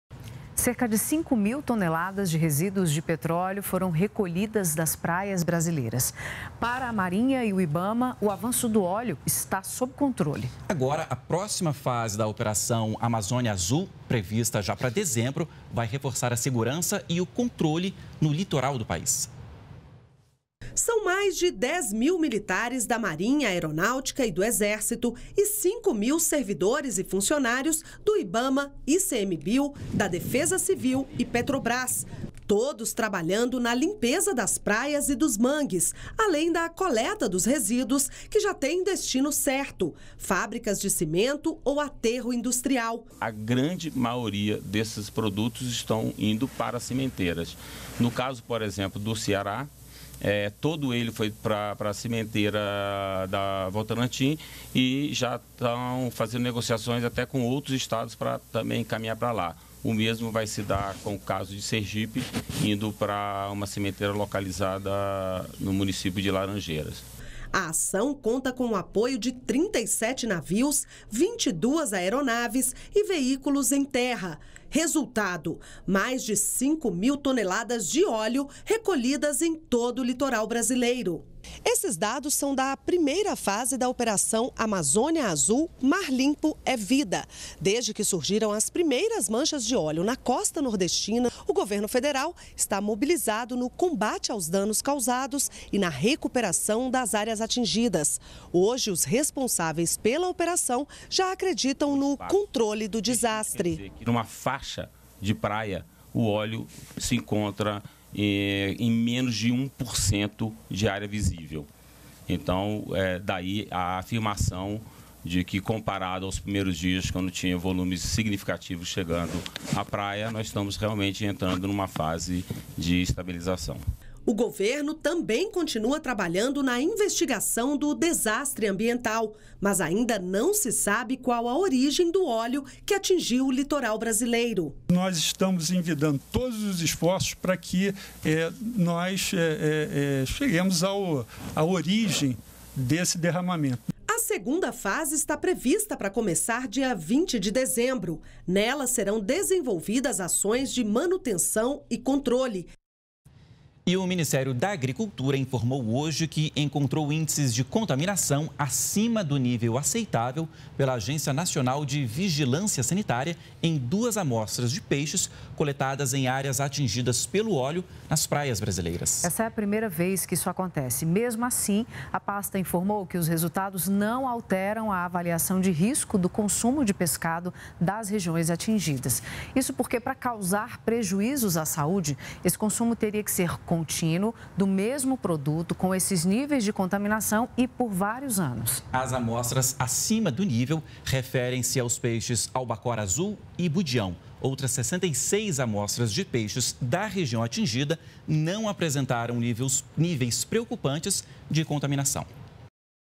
Controle das manchas de óleo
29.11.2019 - Cerca de cinco mil toneladas de resíduos de petróleo foram colhidas das praias brasileiras. Para a Marinha e o Ibama, o avanço do óleo está sob controle. Agora, a próxima fase da operação Amazônia Azul, prevista já para dezembro, reforçará a segurança e o controle no litoral do país.